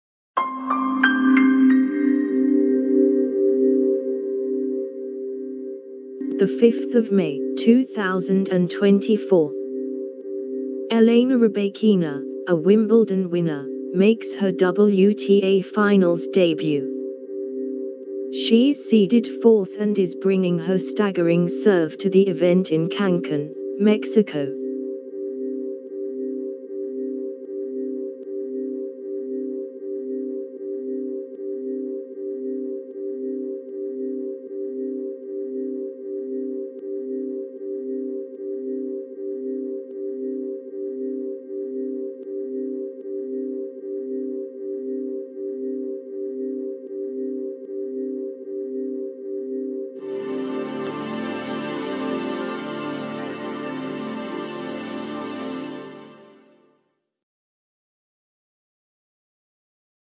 [Latest]Elena Rybakina, a Wimbledon Winner, Makes Her WTA Finals Debut
[Contents]
She’s seeded fourth and is bringing her staggering serve to the event in Cancun, Mexico.